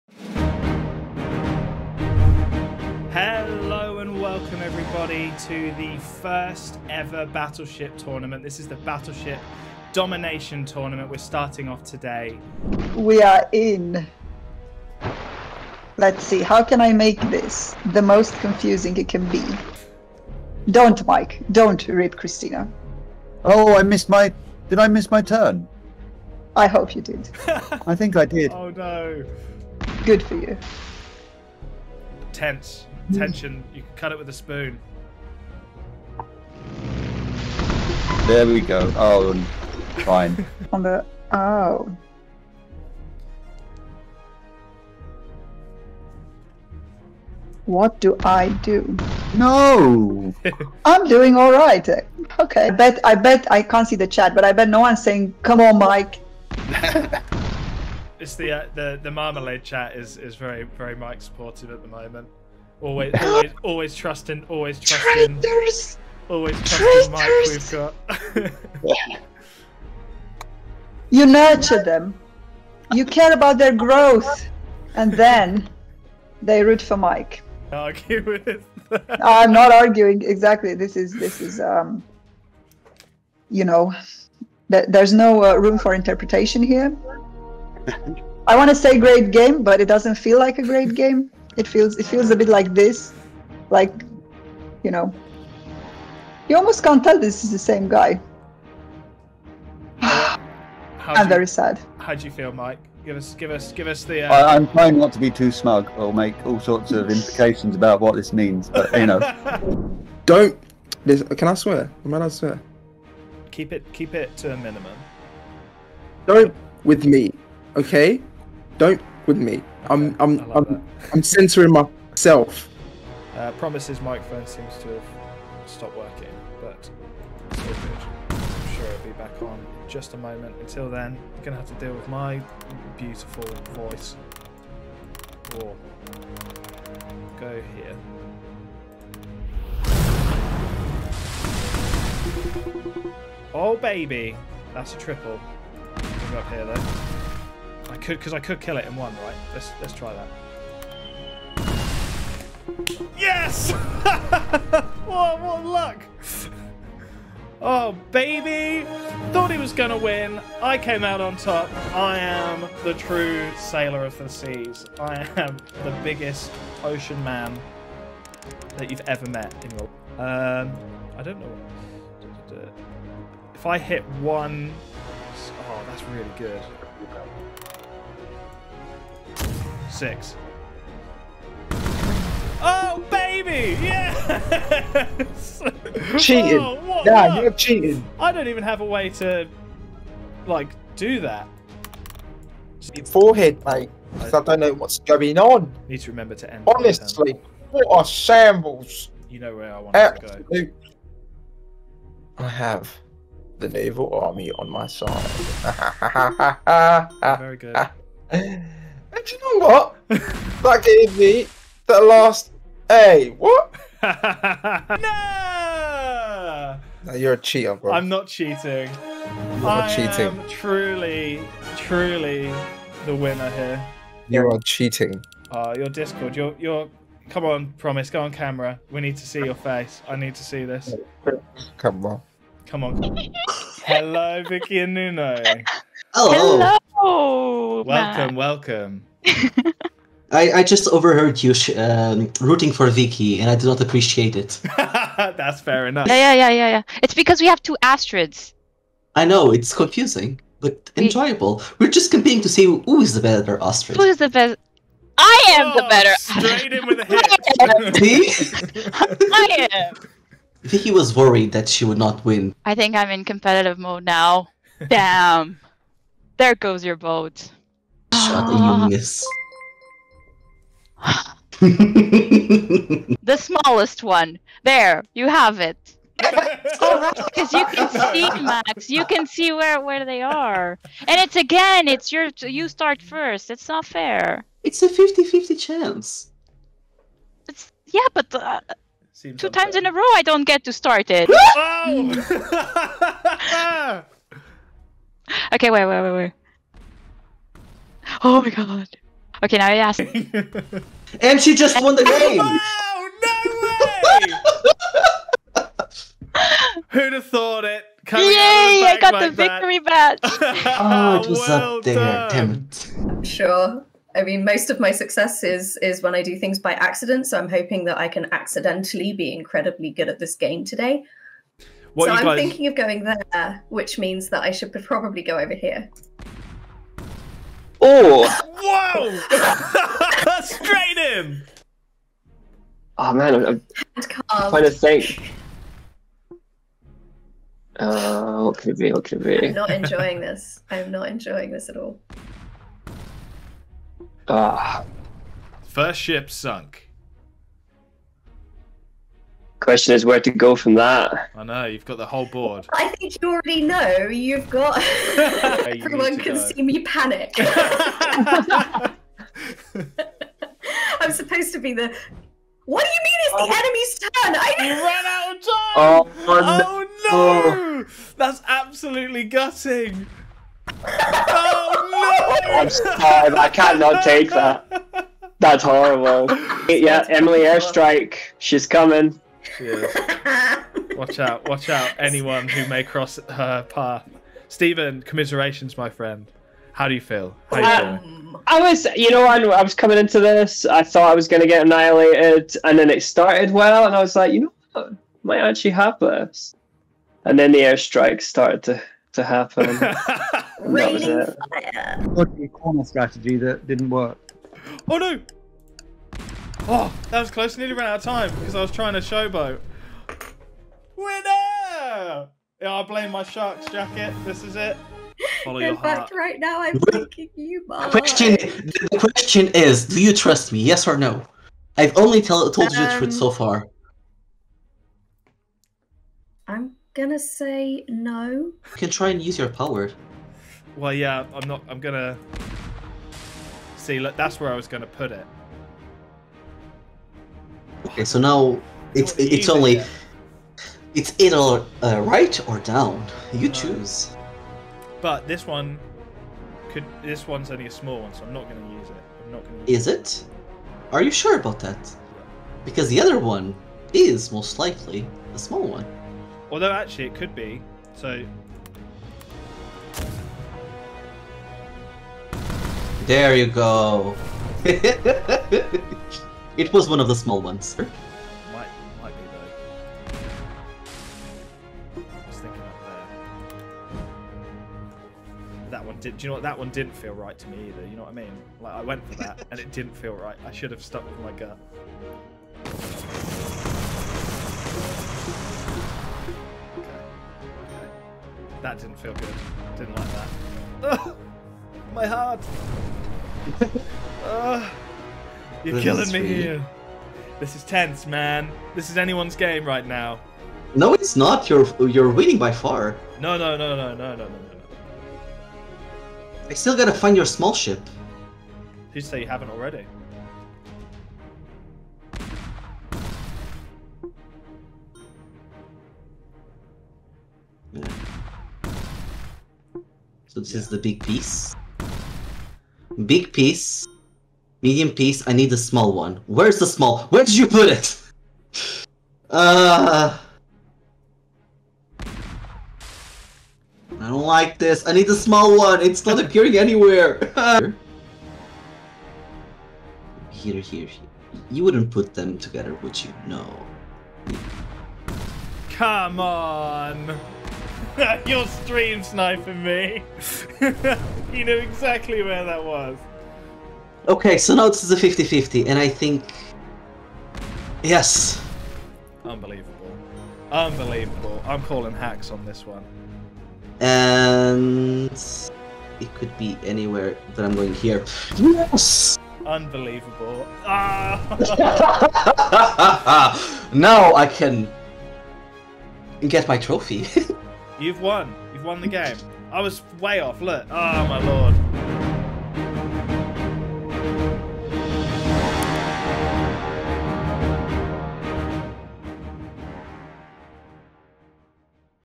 Battleship Domination Best bits
💥 Watch the most exciting moments from our Battleship Domination Tournament. Experience the fun, tension, victories and crushing defeats. It's the classic game, plus high-stakes drama…

Have a great New Year. We’ll see you in 2023!